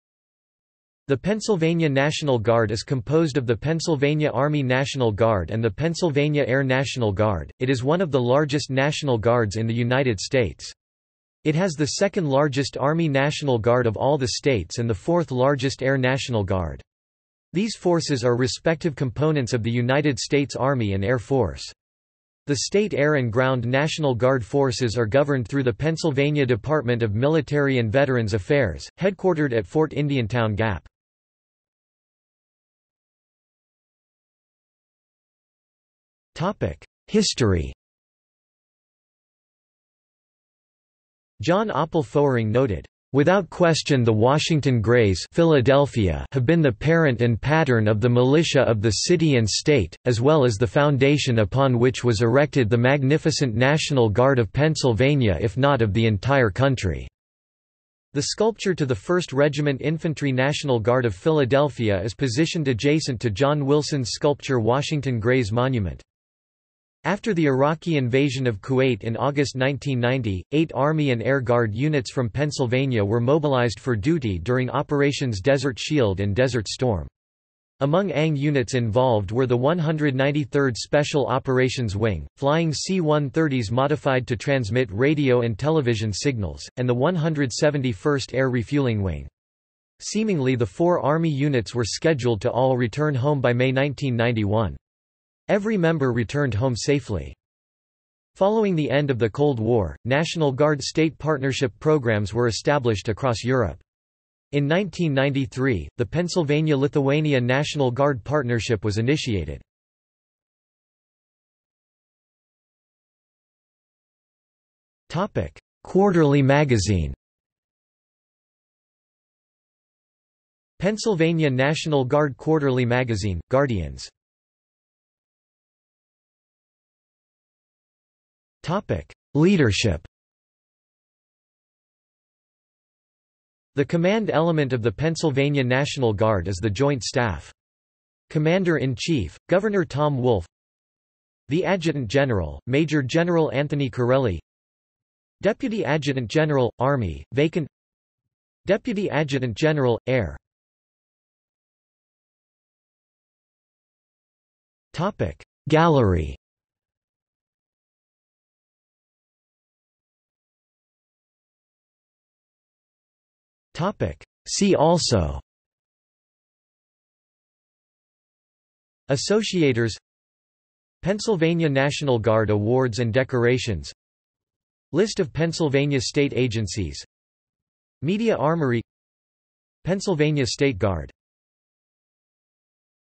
Pennsylvania National Guard | Wikipedia audio article
This is an audio version of the Wikipedia Article:
Pennsylvania National Guard

00:00:32 1 History
00:01:51 2 Quarterly Magazine
00:02:05 3 Leadership
00:02:31 4 Gallery
00:02:40 5 See also

- Socrates

SUMMARY
The Pennsylvania National Guard is composed of the Pennsylvania Army National Guard and the Pennsylvania Air National Guard.  It is one of the largest National Guards in the United States. It has the second-largest Army National Guard of all the states and the fourth-largest Air National Guard. These forces are respective components of the United States Army and Air Force. The state air and ground national guard forces are governed through the Pennsylvania Department of Military and Veterans Affairs, headquartered at Fort Indiantown Gap.